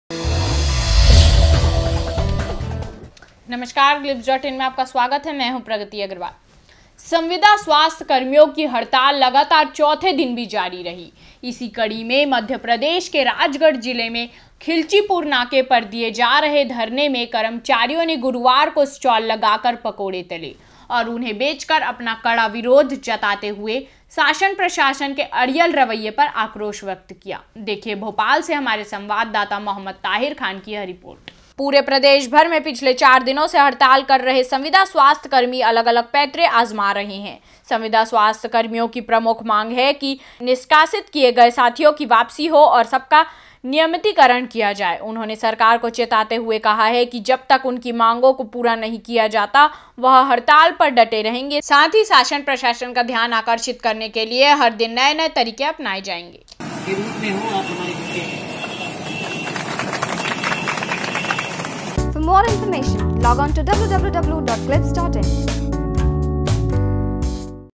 Exclusive: संविदा स्वास्थ्य कर्मियों ने पकोड़े तलकर जताया विरोध
संविदा स्वास्थ्य कर्मियों की हड़ताल लगातार चौथे दिन भी जारी रही,इसी कड़ी में मध्यप्रदेश के राजगढ़ जिले में खिलचीपुर नाके पर दिए जा रहे धरने में कर्मचारियों ने गुरुवार को स्टाल लगाकर पकोड़े तले और उन्हें बेचकर अपना कड़ा विरोध जताते हुए, शासन प्रशासन के अड़ियल रवैया पर आक्रोश व्यक्त किया। देखिए भोपाल से हमारे संवाददाता मोहम्मद ताहिर खान की यह रिपोर्ट